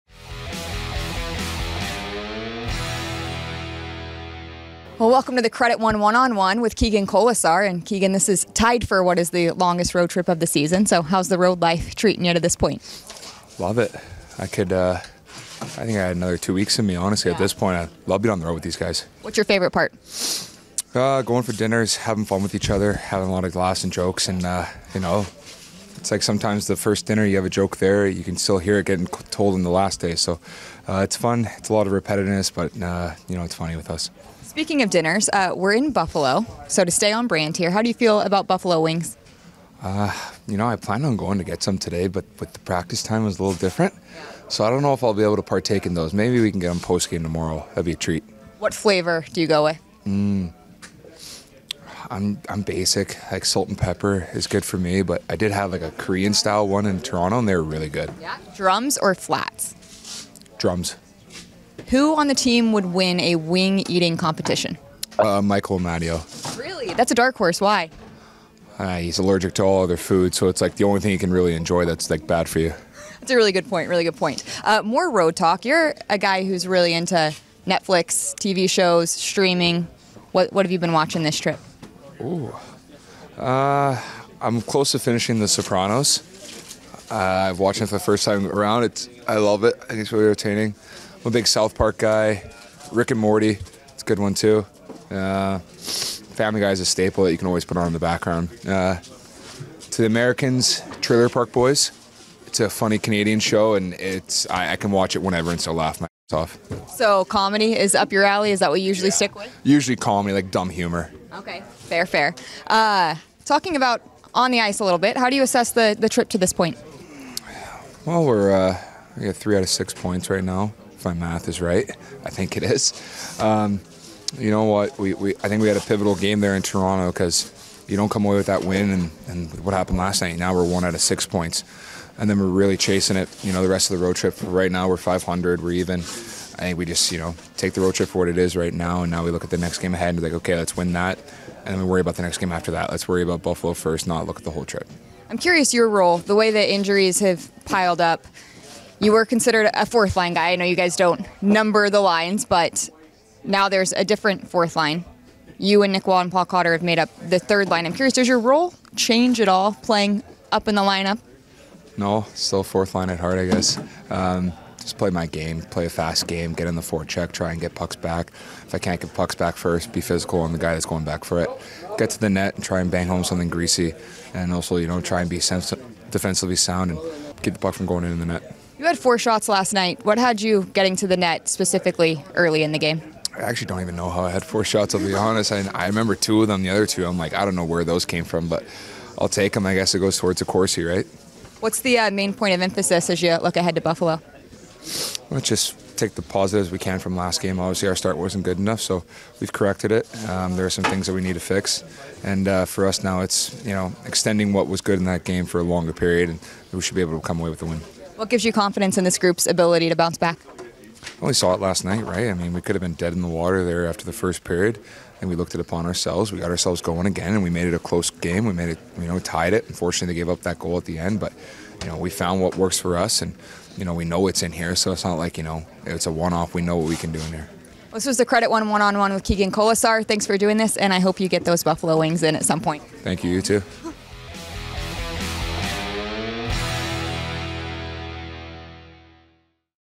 Keegan Kolesar Talks About Life on the Road in the Credit One One-on-One
Keegan Kolesar chatted with Ashali Vise about the lengthy road trip and how the Golden Knights are passing the time away from the rink.